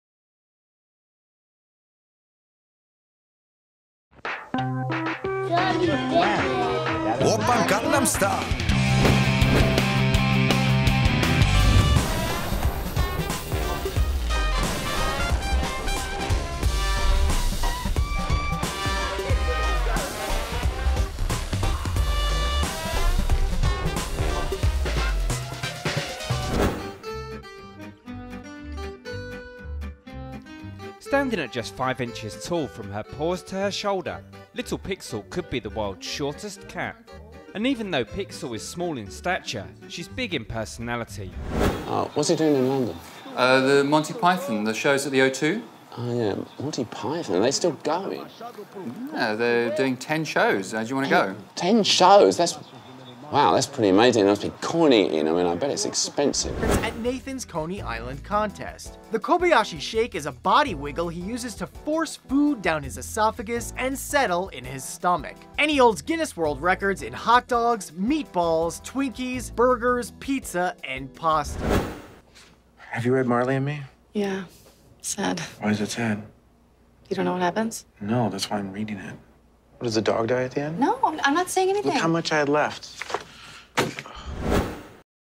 Trending Now 07/01/2014
Super water guns and tiny kittens are among today's trending viral videos.